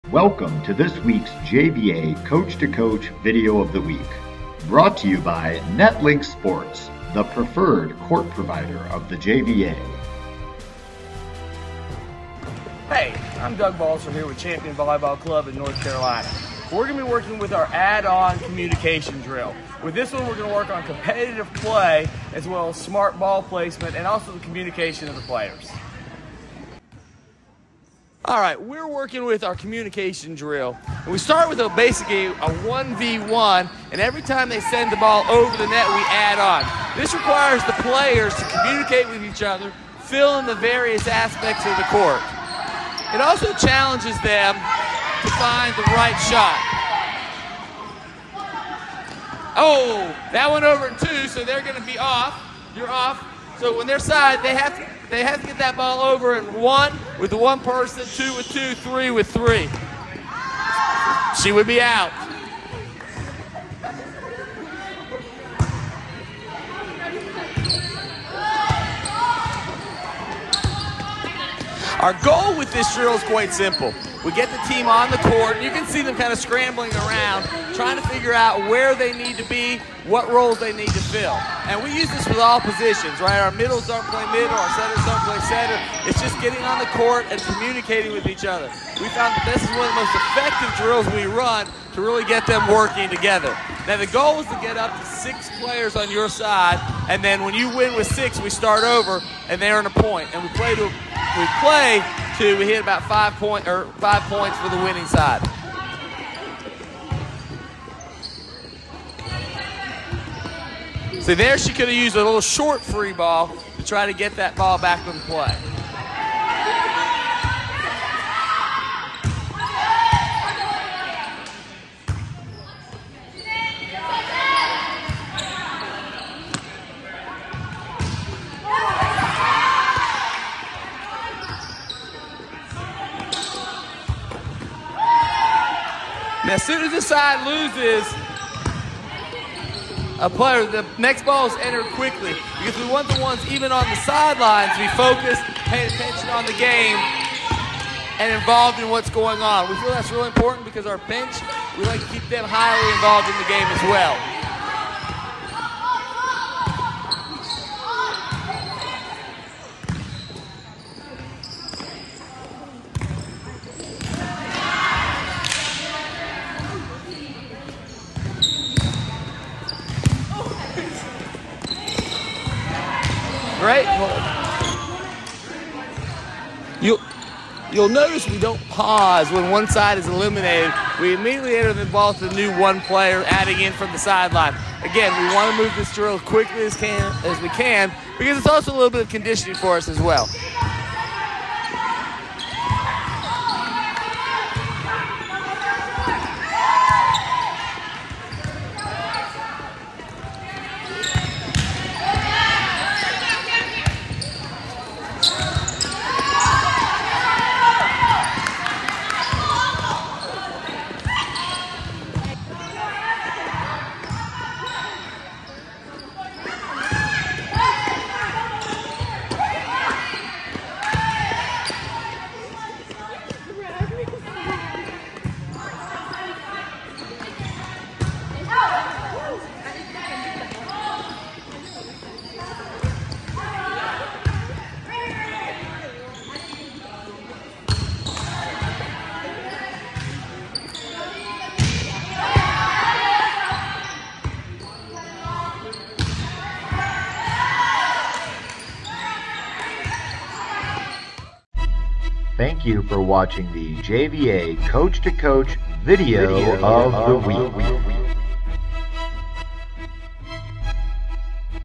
JVA Coach to Coach Video of the Week: Add on Communication Drill
Champion Volleyball Club (Winston-Salem, NC) shares a fun warm up drill that focuses on communication, competitive play, and volleyball IQ). The objective is for one side to get 6 players on the court and win a point, first team to do that 3 times or 5 times wins!